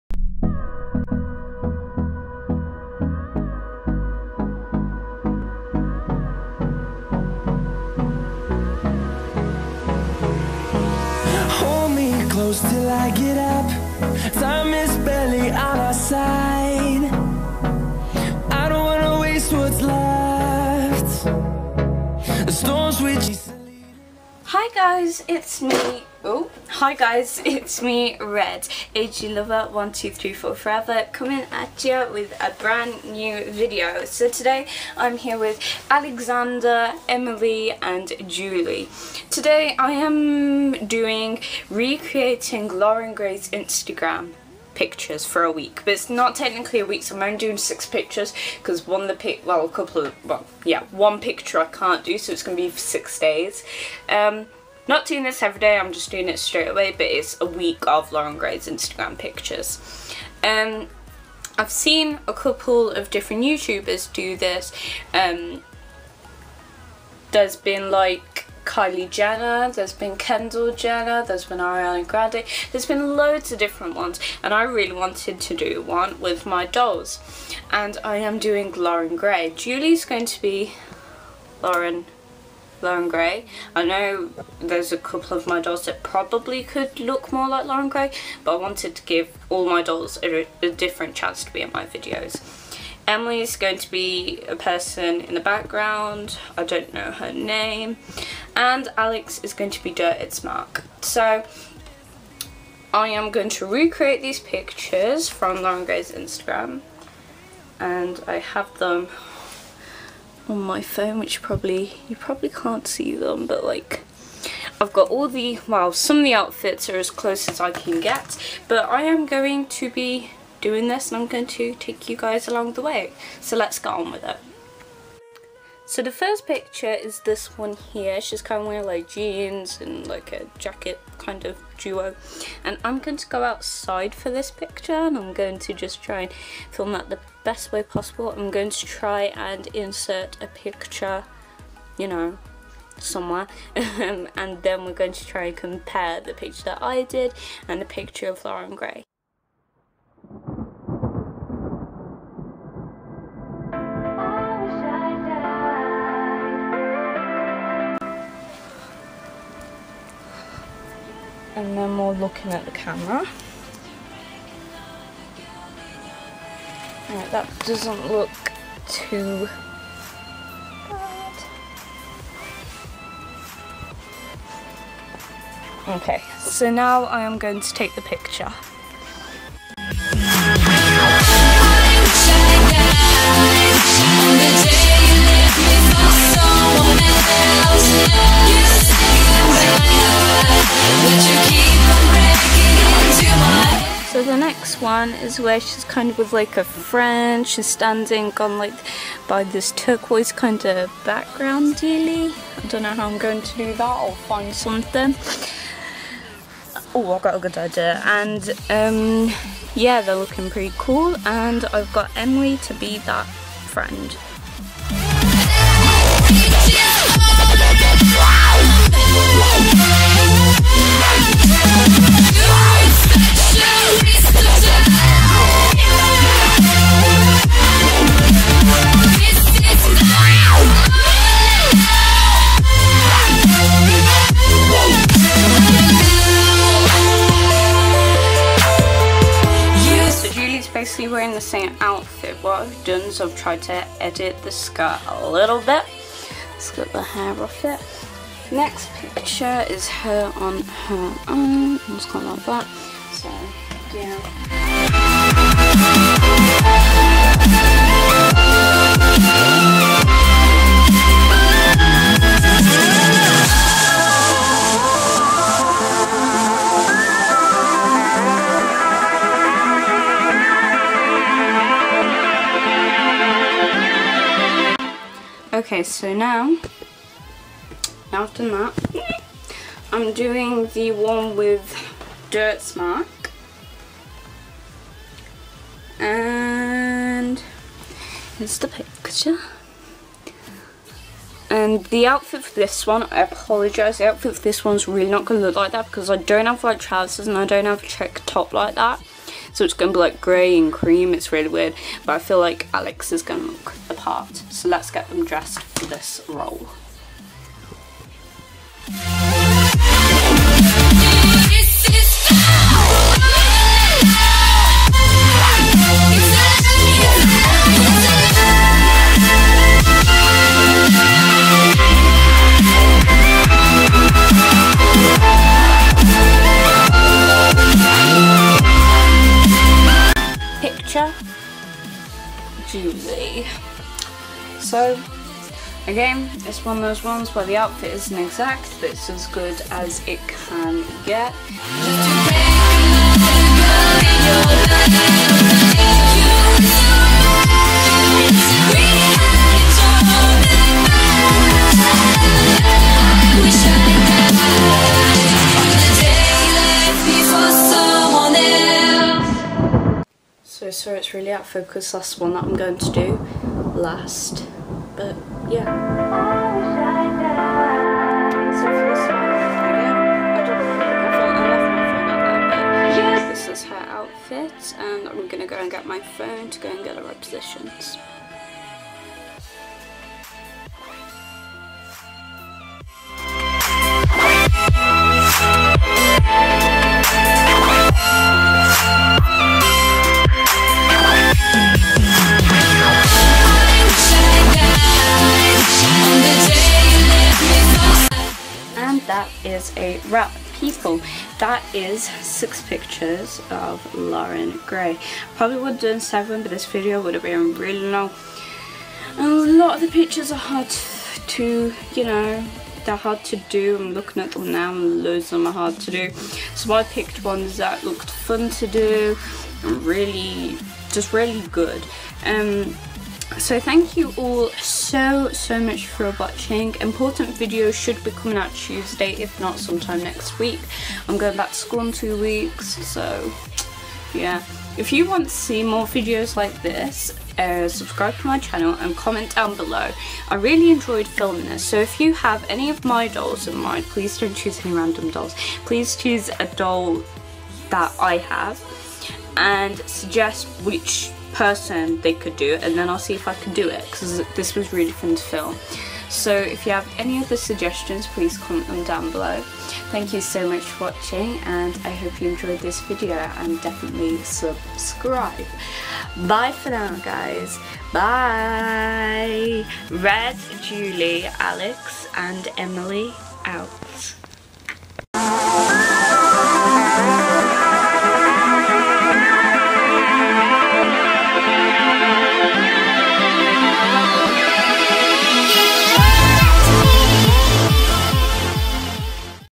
We Copied Loren Gray's Instagram Posts For A Week! AGLF American Girl!
LOREN GRAY PHOTO RIGHTS!!!
Hi guys! please bare in mind that these are dolls and the pictures would of been better if humans were doing the photos :)